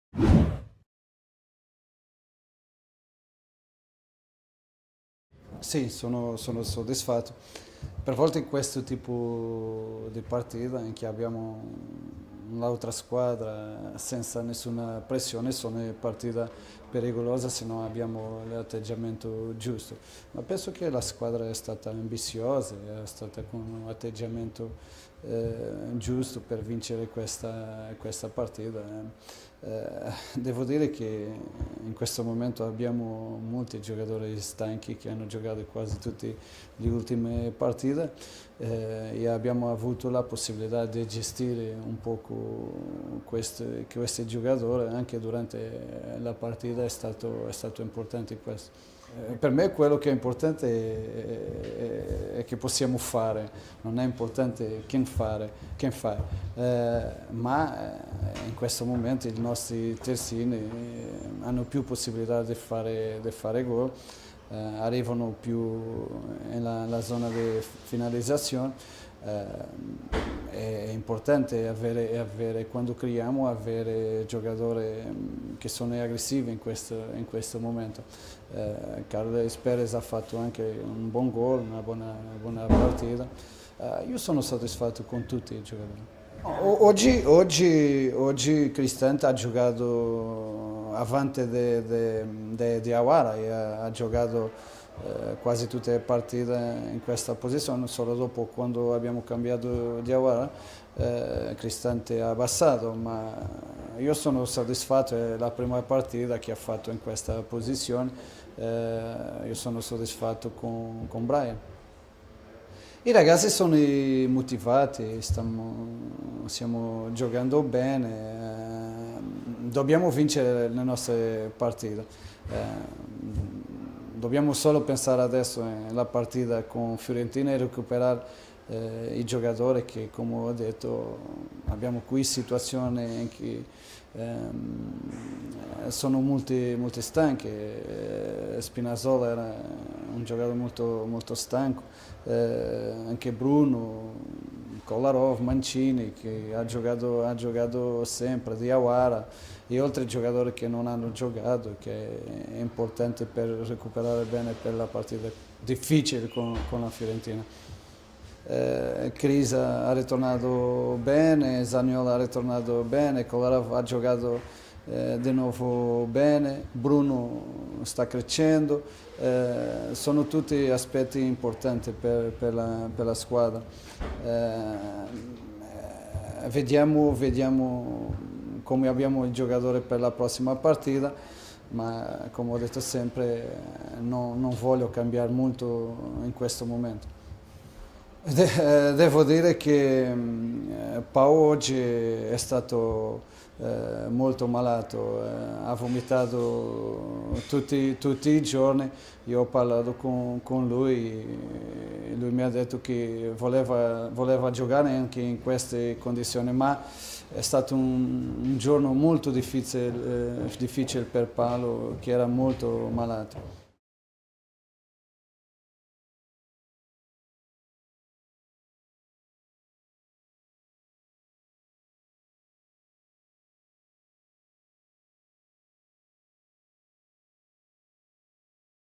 INTERVISTA POST PARTITA PAULO FONSECA | SPAL 1-6 Roma | ZonaGiallorossa
intervista completa Paulo Fonseca nel post partita di SPAL Roma finita con la vittoria 1-6 per i giallorossi.